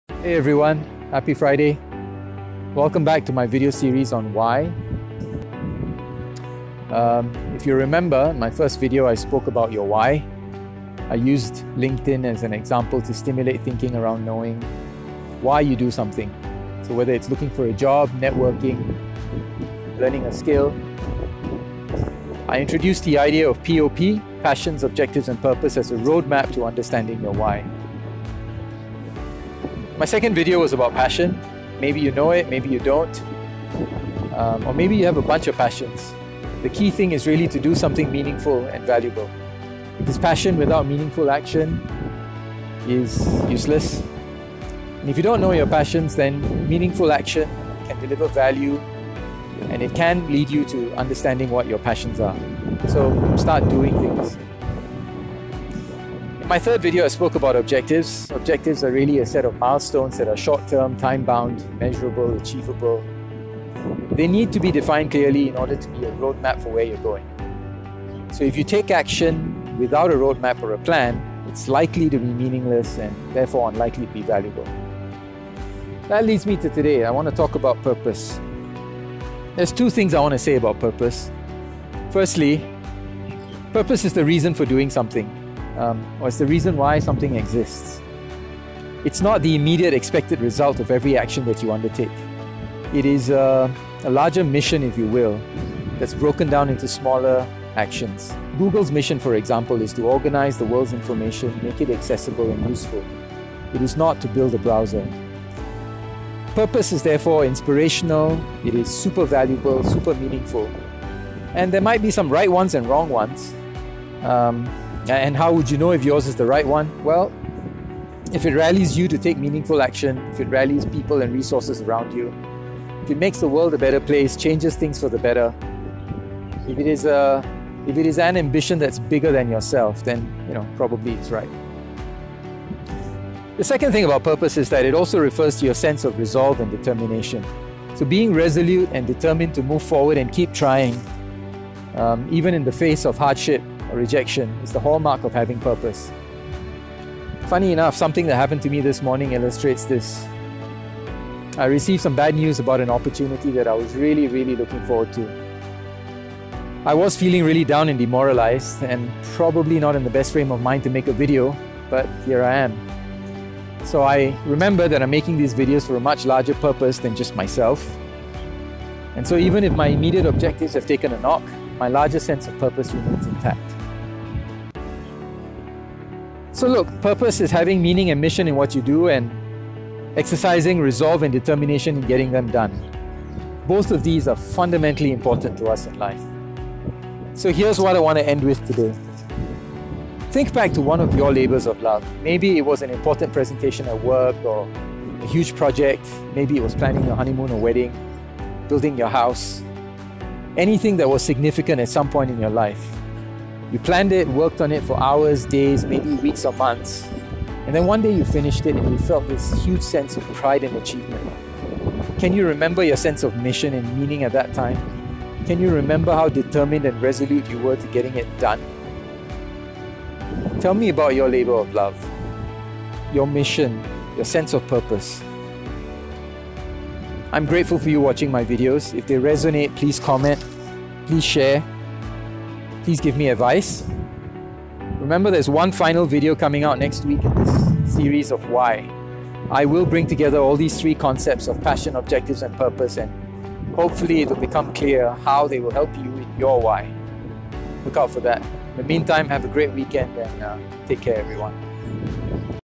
Discovering Your Why - Passion-Objectives-Purpose. This one is about "Purpose".
In this video I focus on "Purpose". What is your mission? It needs to be meaningful. I consult with individuals and businesses in the travel domain - please for more.

This is the 4th video in the series - "Your Why". The first was an intro to "POP" - Passion, Objectives and Purpose. The 2nd was about Passion; 3rd about Objectives and this one about Purpose. It is the reason why something exists, It's not just the result of every action. It's your MISSION. It should be inspirational - rally people and resources around it. It is an ambition bigger than yourself. Purpose is also about resolve and determination; they keep you going even in tough times. It feeds off your MISSION. It is usually a labour of love.